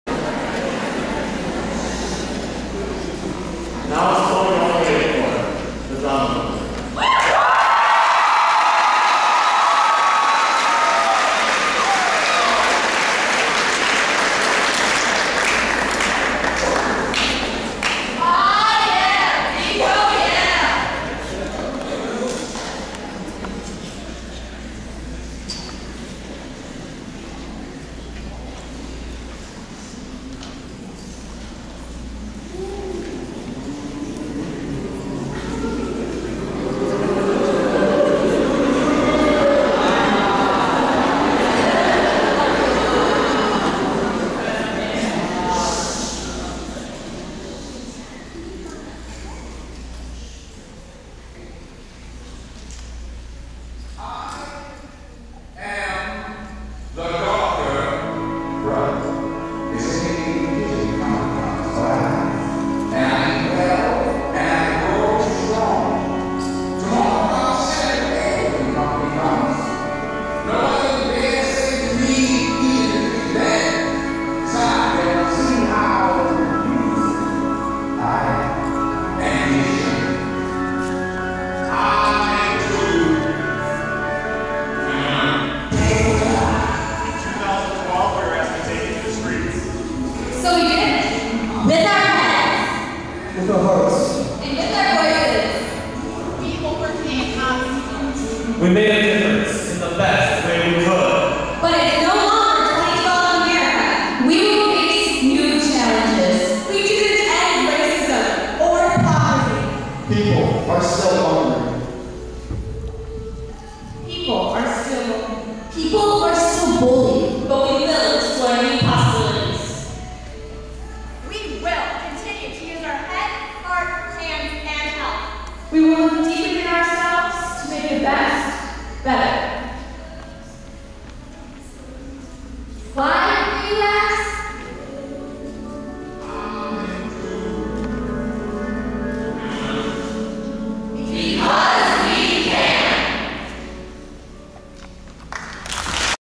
2013 Wisconsin State 4-H Drama Company ( opening 1)
The 2013 State 4-H Drama Company is comprised of over thirty youth members. That live throughout Wisconsin.   These older youth gather together to create a unique  theater performance as well as offering performances that showcase their instrumental, vocal and dance talents. The theme of the 2013 4-H Youth Leadership Conference is "Exploring the Possibilities".   Most of the performing arts performances are hosted at the Madison Masonic Center.    Drama Company works an average of 11 hours a day to create and perfect a theater performances which exudes professionalism as is created through collaboration and dedication by every member and the supporting staff of professional directors and volunteer ad visors.

Theater Directors: Jay Johnson and Pat Henderson
Adult Youth Advisors: Rick Snyder,Barb Satnik,Catherine Fry and Susan kloch Wendlandt
Videography: Susan Kloch Wendlandt